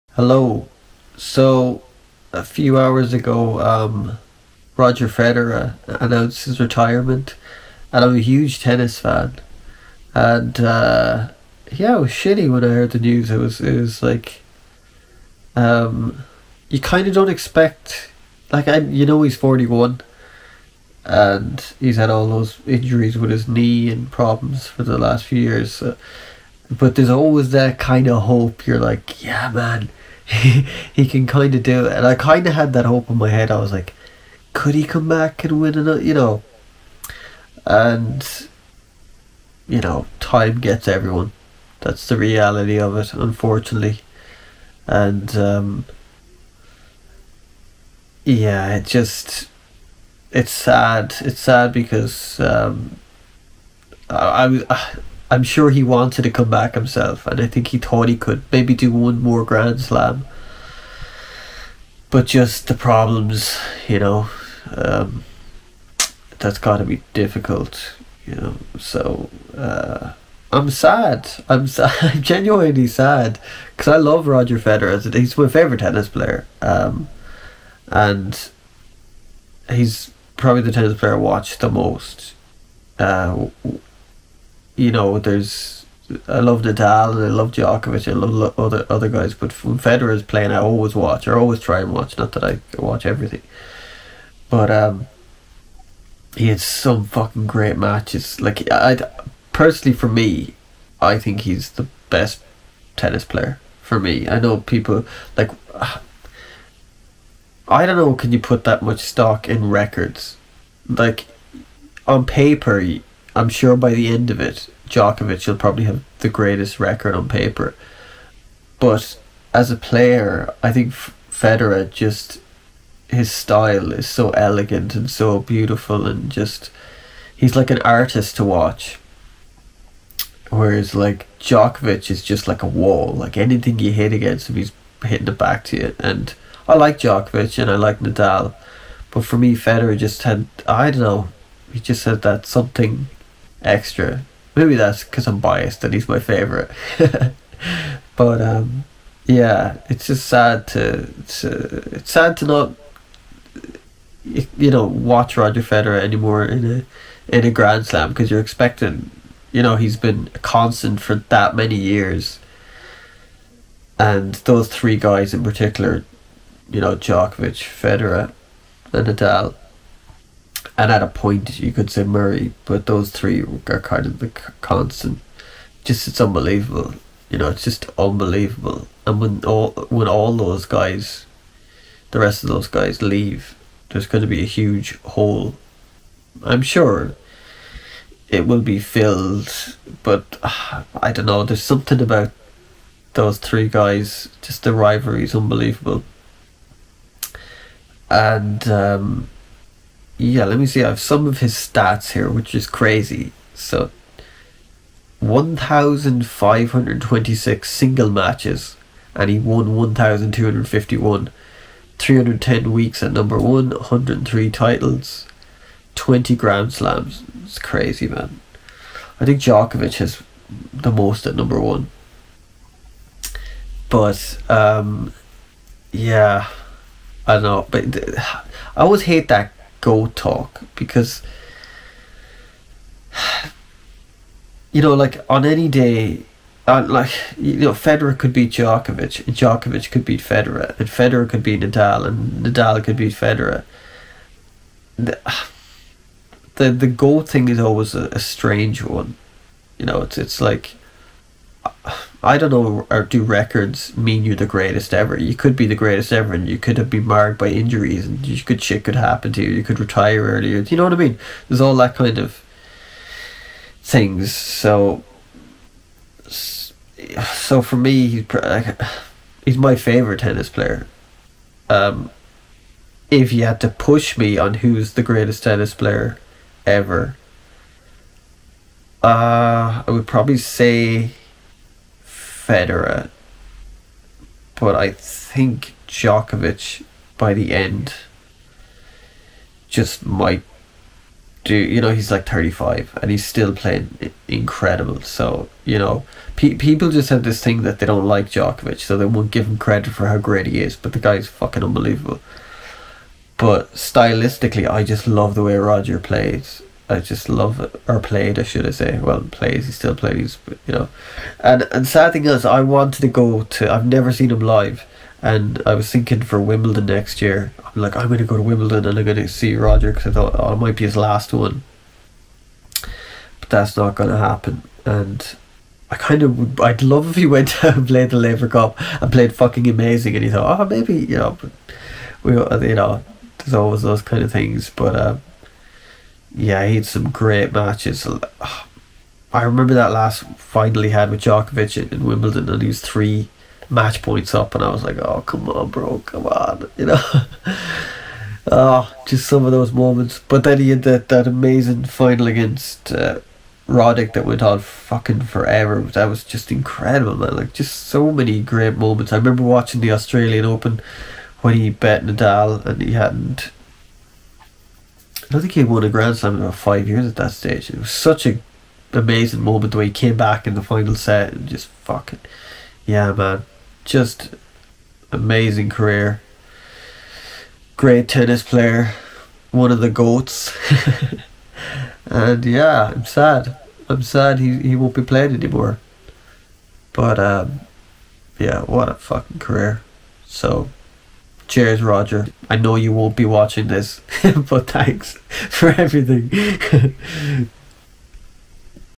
Roger Federer retires
One of the GOAT's of Tennis Roger Federer retires from Tennis. What a career. Sad to see my favorite player retire. Here are some words.